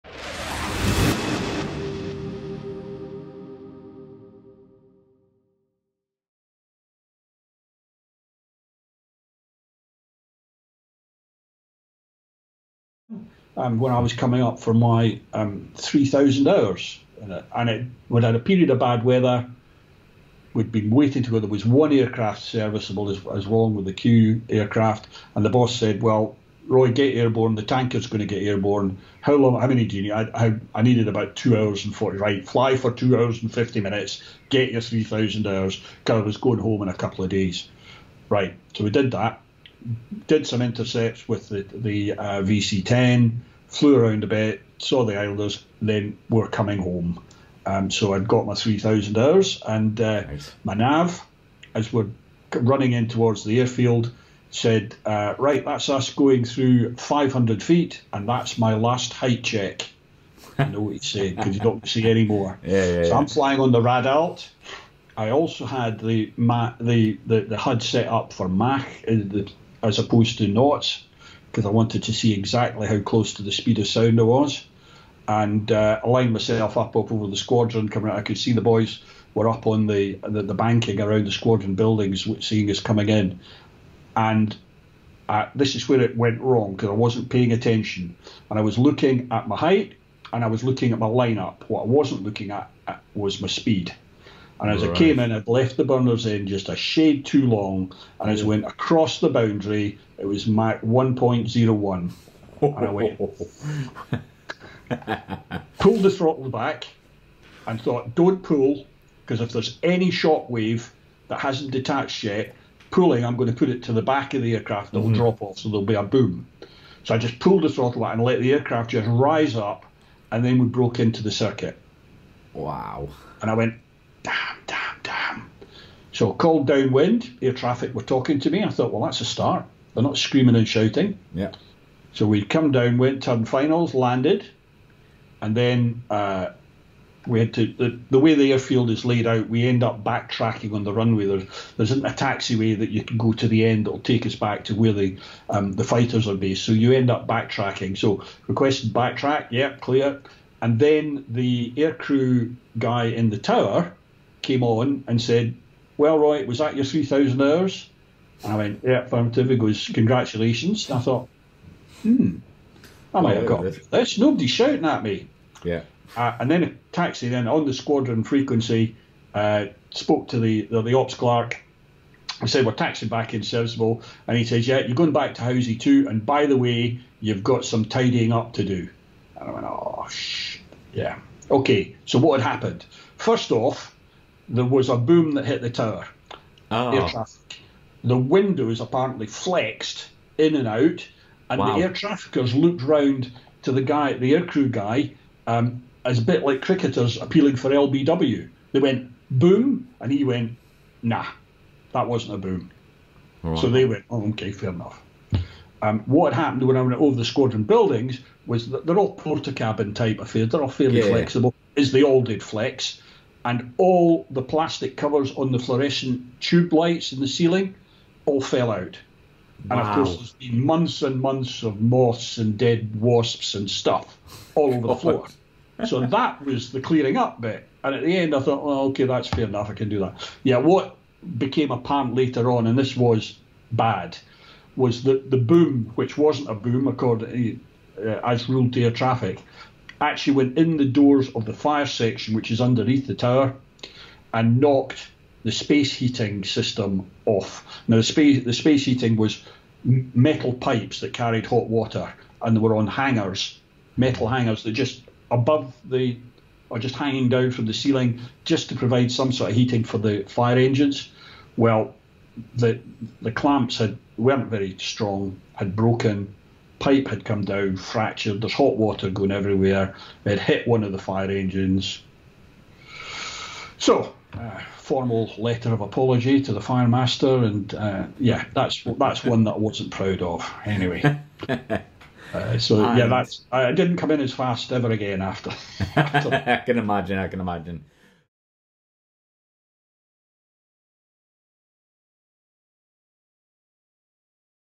Supersonic F3! | Roy Macintyre Gets His 3000 Hours
Former Tornado F3 pilot, Roy Macintyre shares the great supersonic story when he got his 3000 hours in the Tornado F3!

Purchase our Aviation Art Book, Volume One